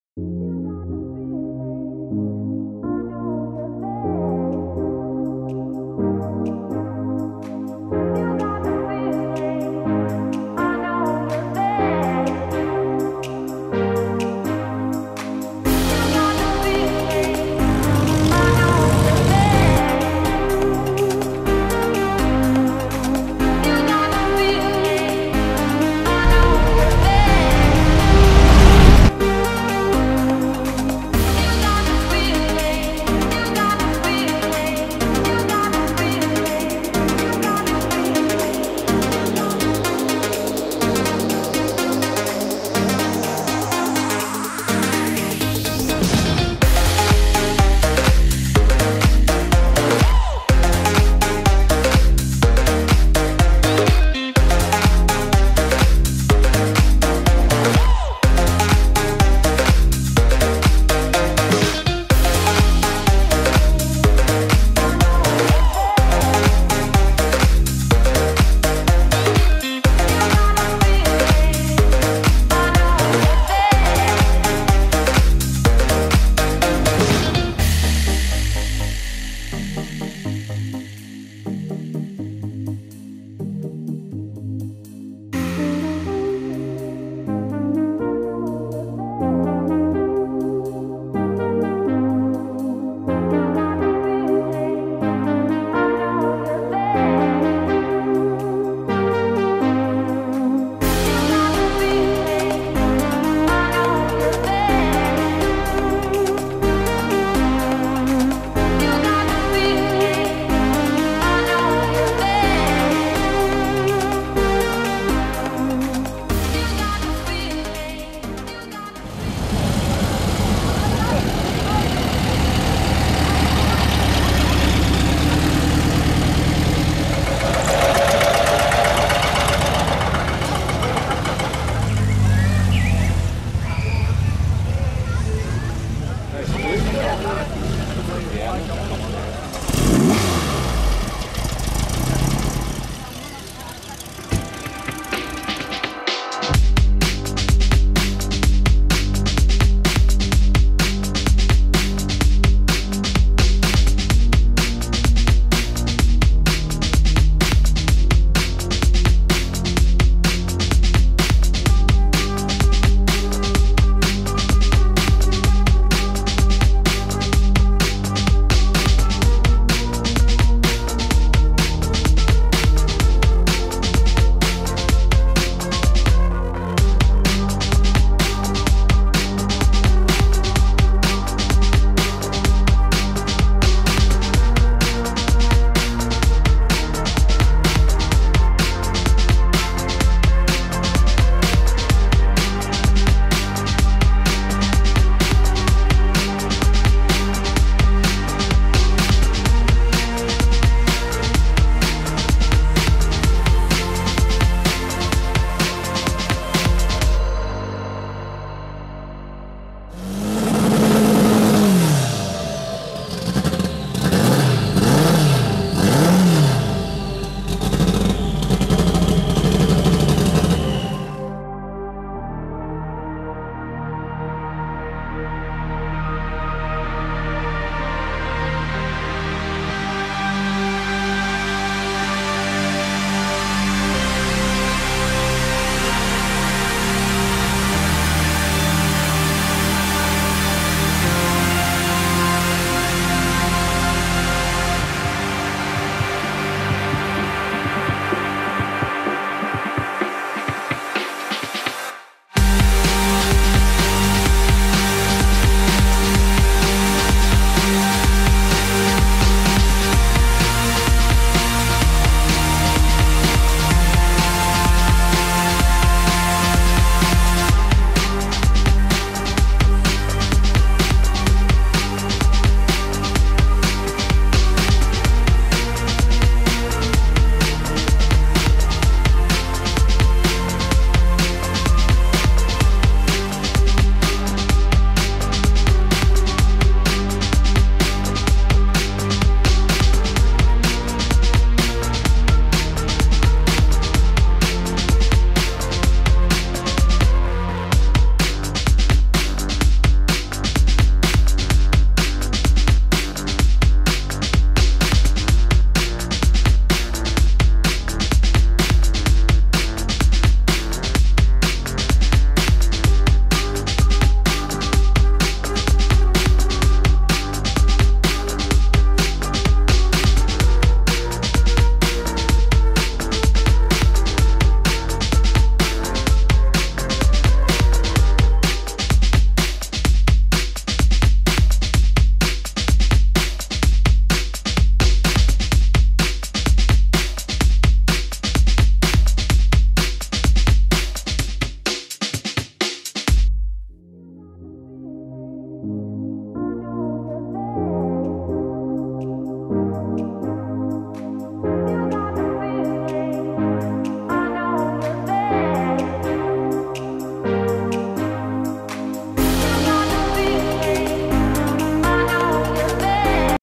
Bugs, Beer & Badeanstalt l Aircooled VW Bug Show 2018 Around a pool
VW Bugs Around a pool l Aircooled Show l An unusual beetle meeting with air-cooled VWs and Porsche, into the outdoor pool to Oelde-Stromberg,  ein ungewöhnliches Käfertreffen, mit luftgekühlten VWs und Porsche, ins Freibad nach Oelde-Stromberg.